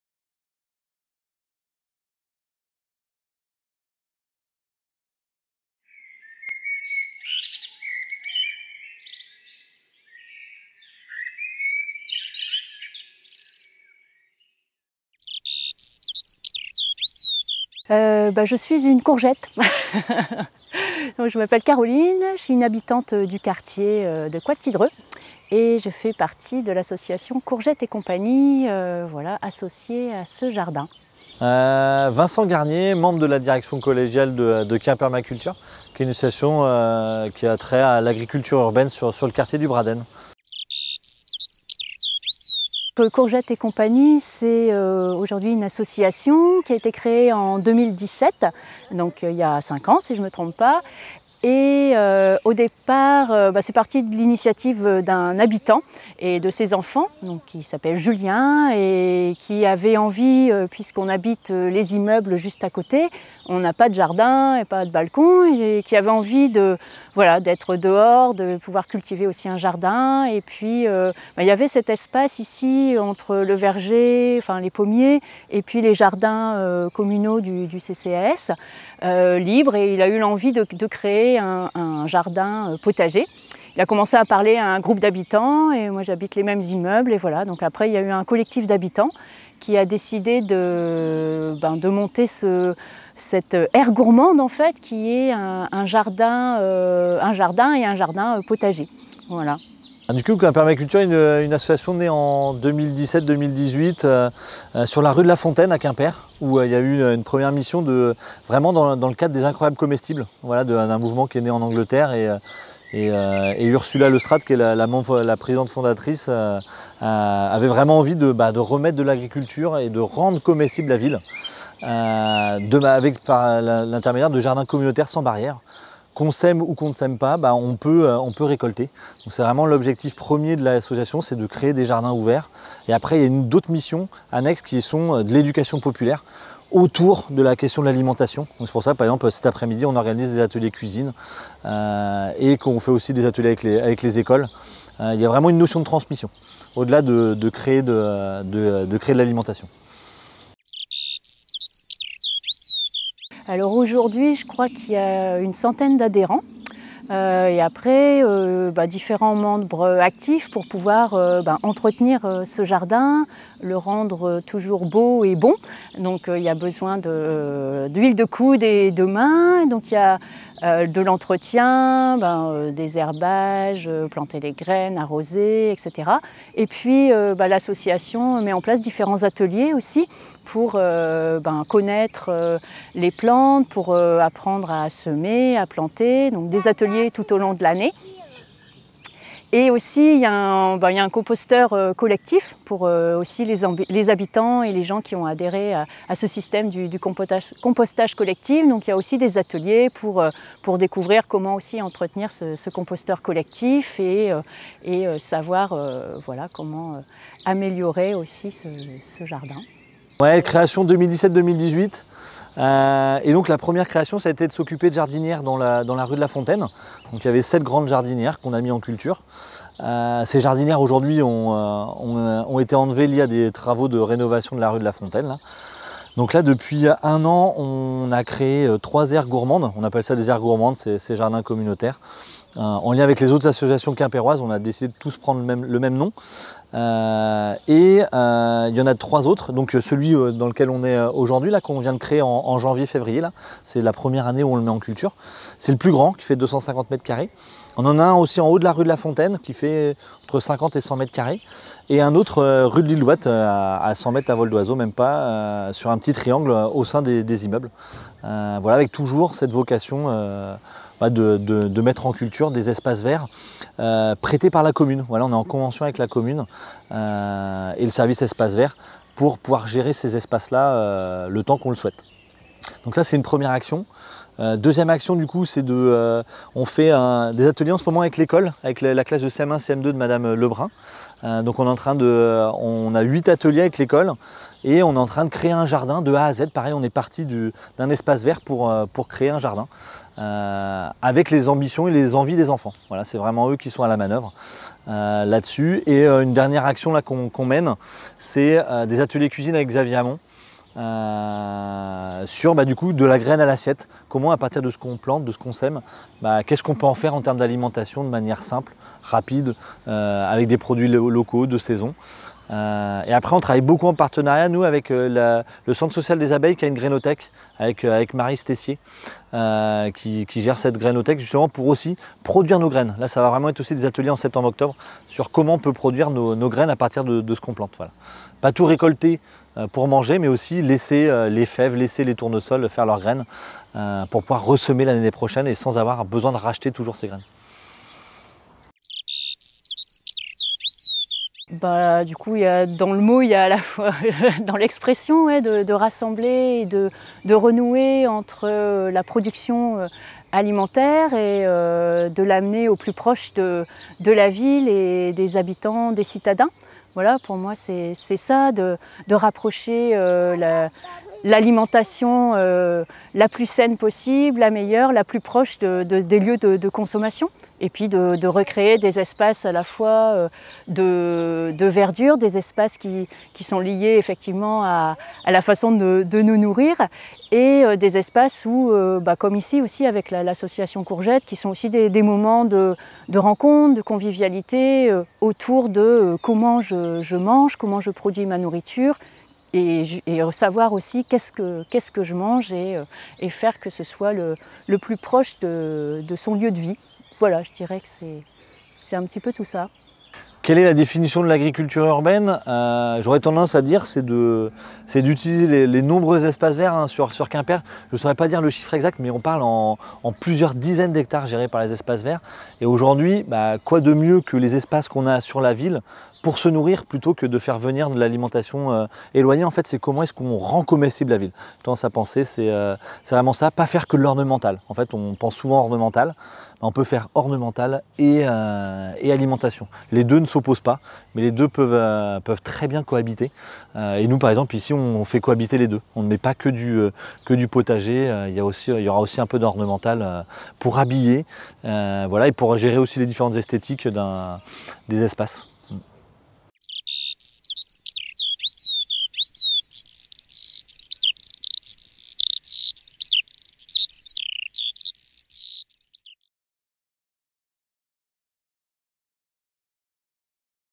Interview croisée : courgette et Cie et Kempermaculture
Courgettes et compagnie et kempermaculture ont mis en place des aires gourmandes dans deux quartiers de Quimper.
Caroline et Vincent sont venus nous parler de leur association respective et leur définition de l'agriculture urbaine.